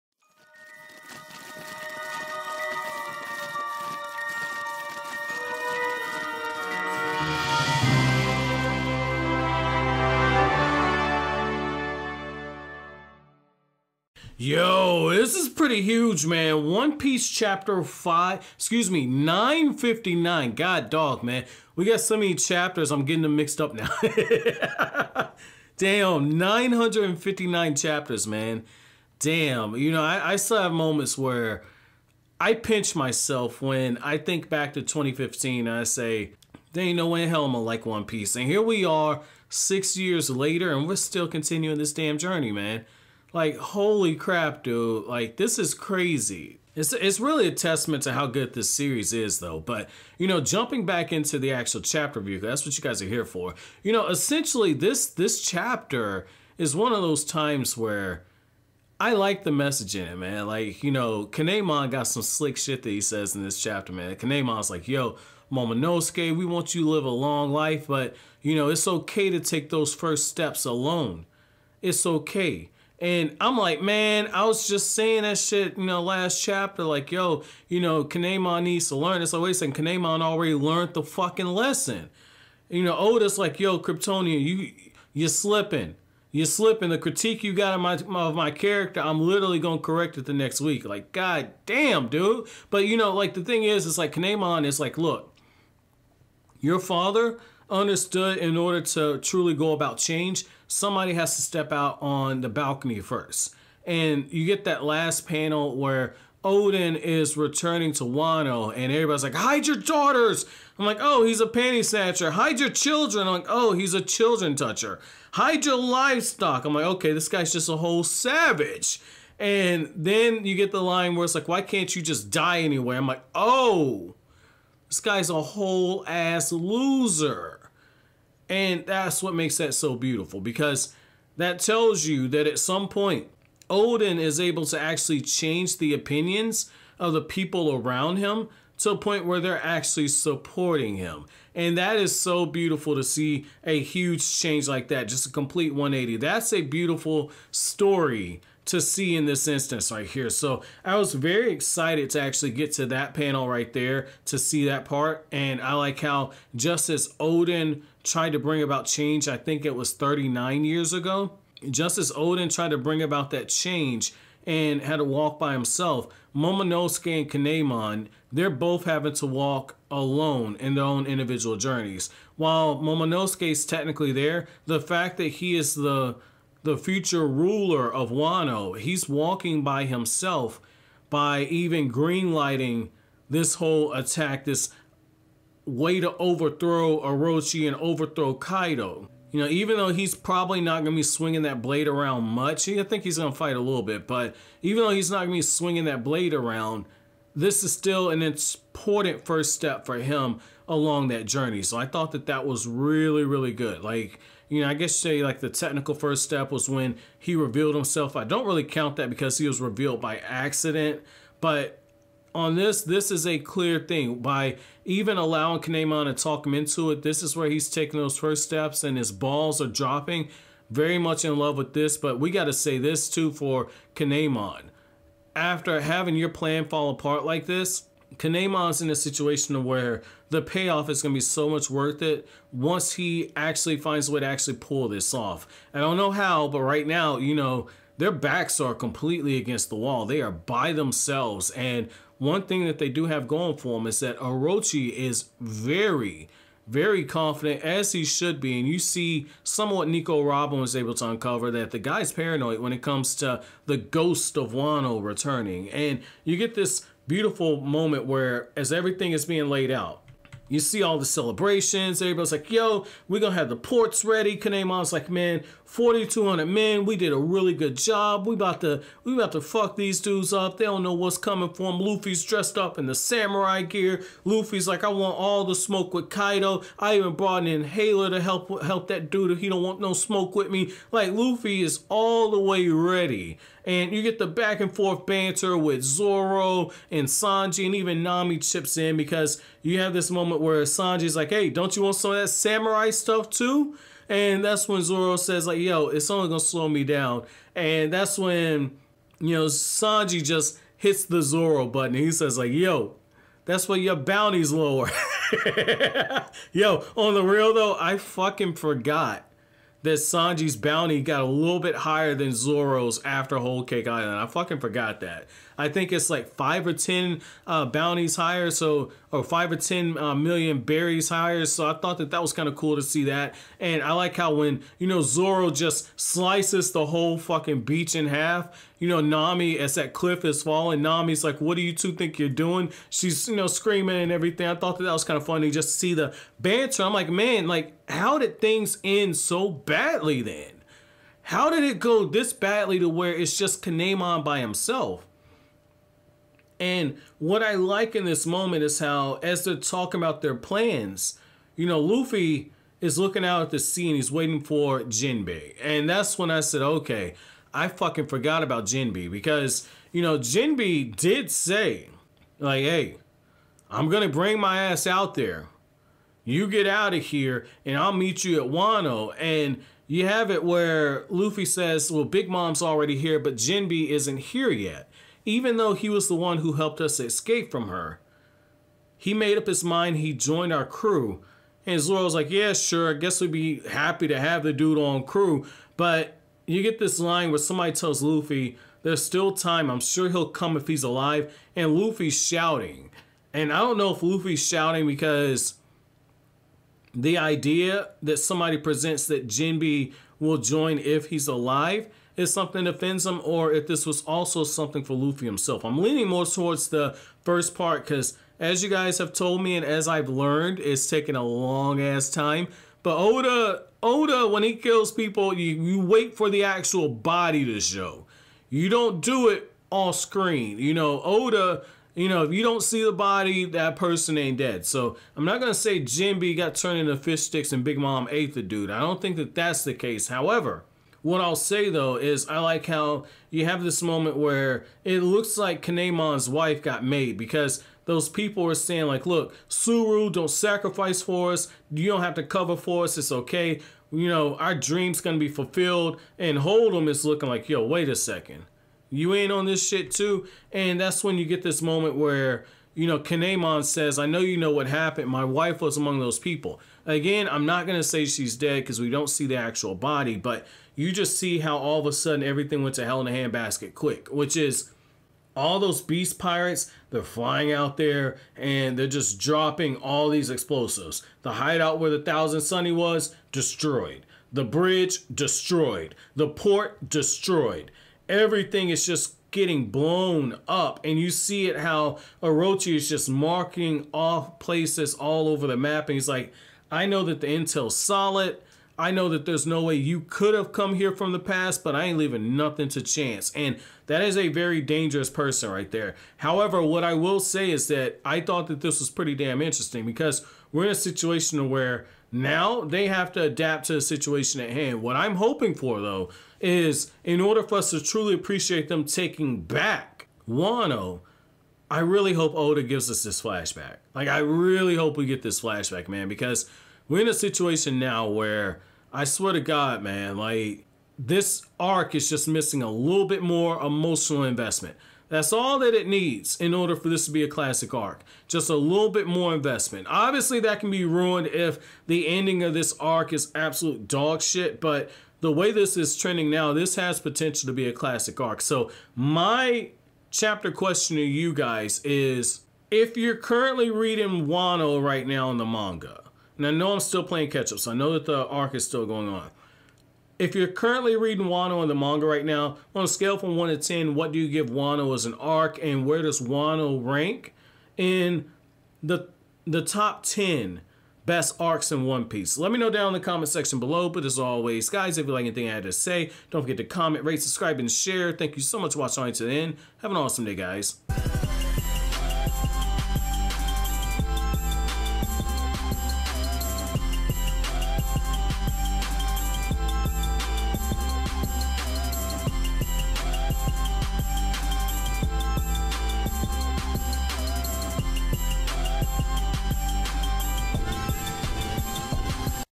One Piece Chapter 959 Review~Samurai
In the days prior to the Fire Festival, Orochi had ordered his men to destroy the bridges connecting the country's regions in order to prevent the Kozuki alliance members from convening on Port Tokage. He also had his men attack the Thousand Sunny after finding out where it was, and in Ebisu Town, the citizens drew Holdem's wrath after claiming to have eaten the food rations that the Nine Red Scabbards stole. In the present day, the Scabbards decide that they must go forward with their plan no matter what, remembering that Oden told them the tides of the world would only change once Wano's borders are opened.